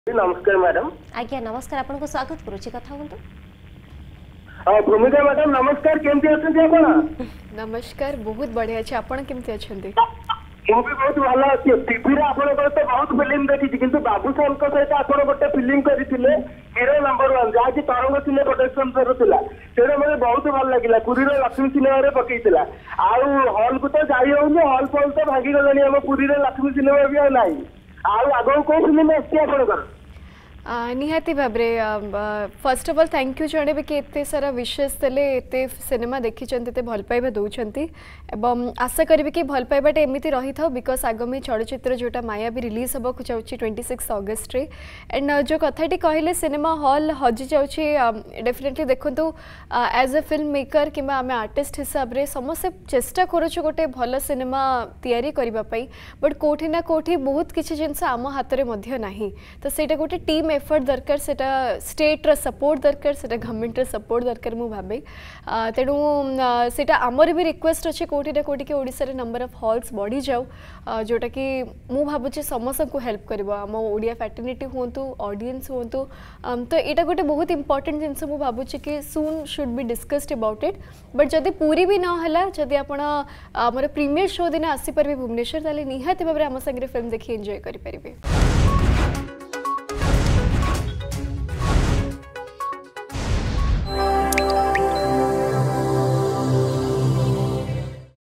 "ମାୟାବୀ" ସିନେମାରେ ଭୂମିକାଙ୍କ ଦମଦାର ଅଭିନୟ
ଶୁଣନ୍ତୁ ଓଡିଆ ସିନେମାର ଭବିଷ୍ୟତ କୁ ନେଇ କଣ କହୁଛନ୍ତି ଅଭିନେତ୍ରୀ ଭୂମିକା ଦାଶ ।